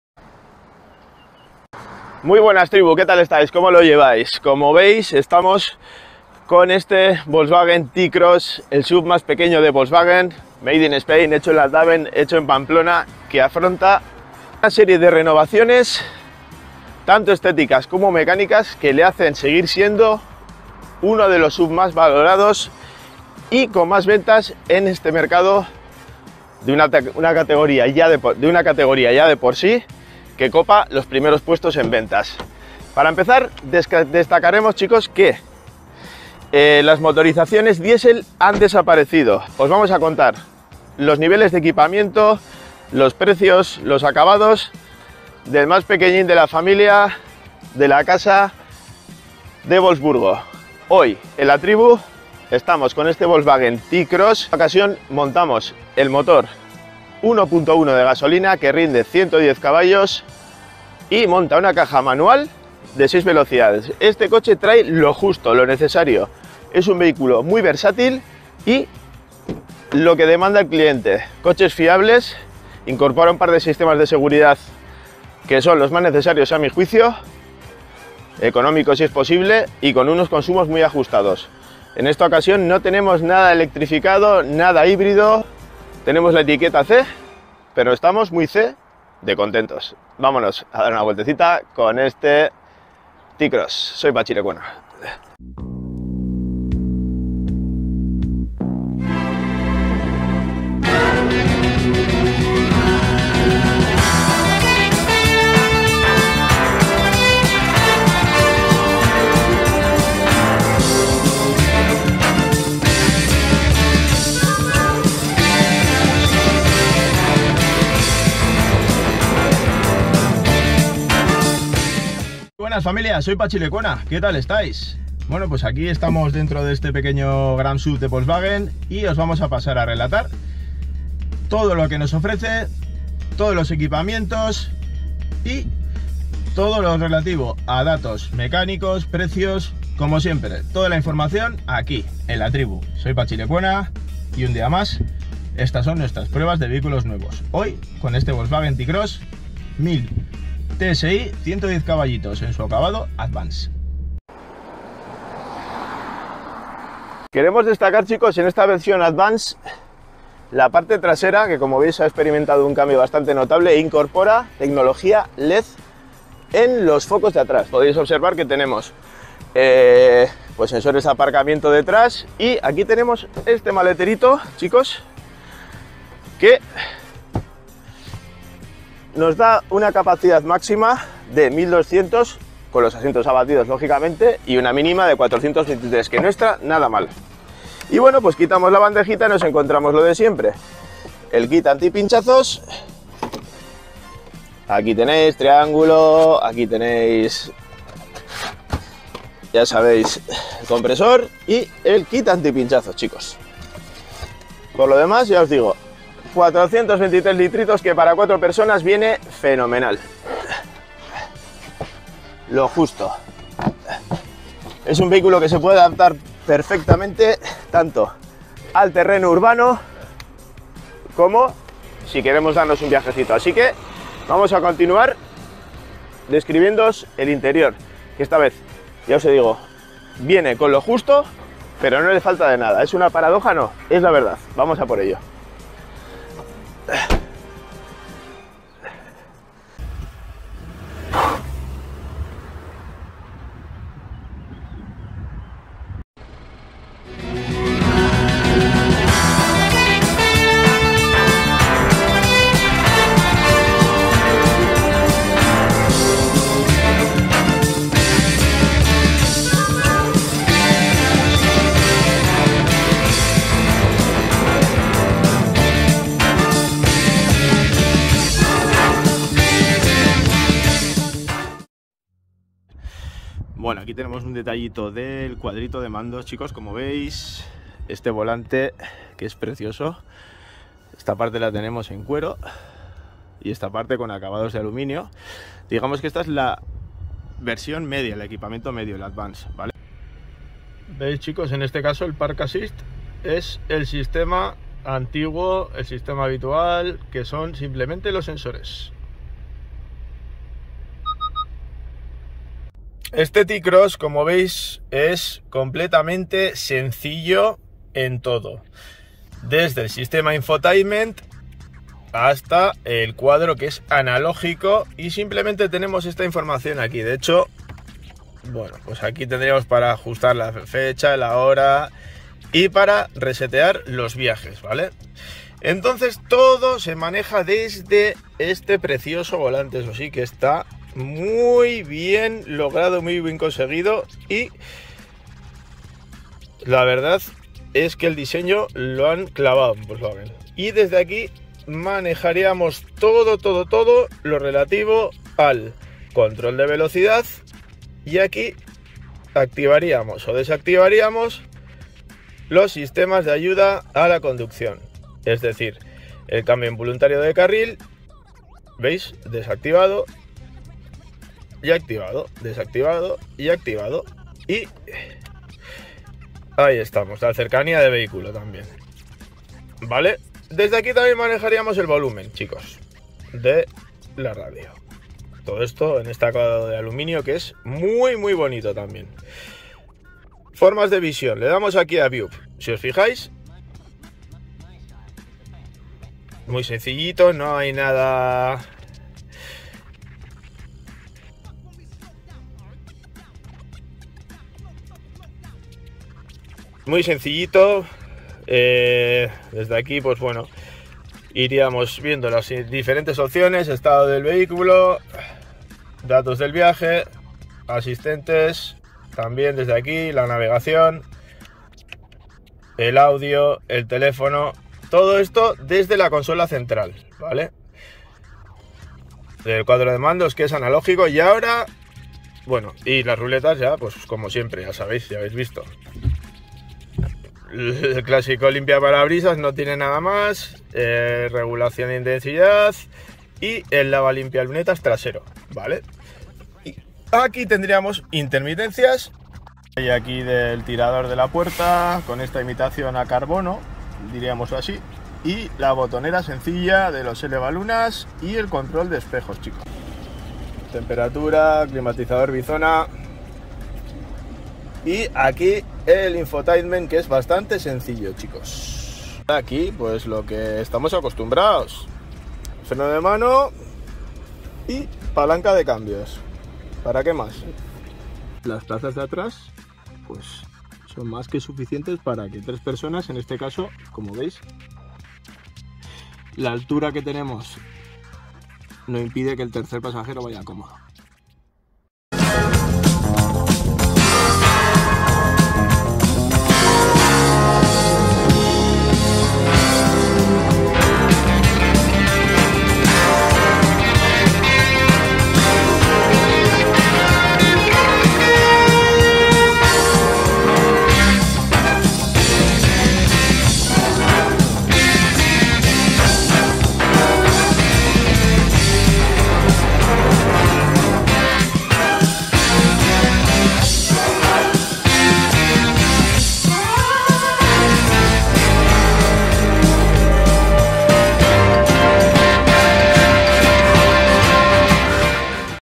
Volkswagen T-Cross prueba completa (1a parte)
0:00 Presentación
3:00 Interiores y descripción detallada
8:00 Cierre
La gama ha sufrido importantes ajustes en 2021. Las mecánicas diésel se han retirado y ahora solo es posible configurar el SUV más pequeño de Volkswagen con motor de gasolina. Listamos todas sus  versiones y repasamos el equipamiento más destacado del Volkswagen T-Cross , probamos el paquete Advance que monta un mtor 1.0 tsi con 110 cv de potencia ¡¡¡ No te pierdas la prueba completa  de este pequeño gran Suv !!!